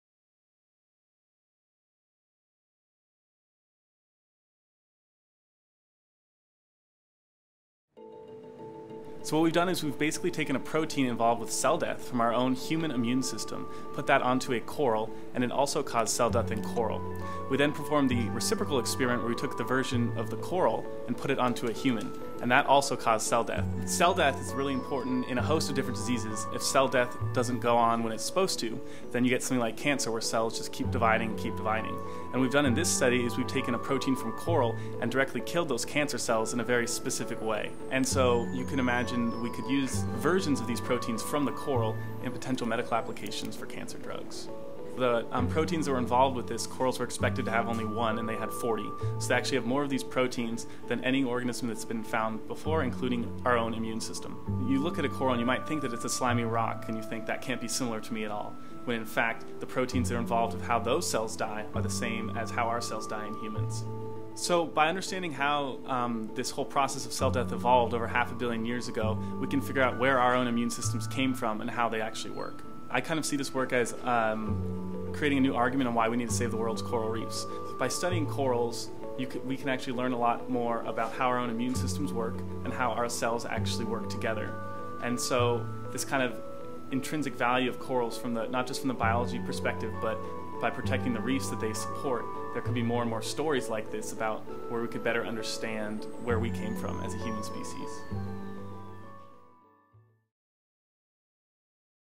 Cellular Self Destruction
Humans and corals are about as different from one another as living creatures get, but a new finding by SDSU graduate student Steven Quistad reveals that in one important way, they are more similar than anyone ever realized.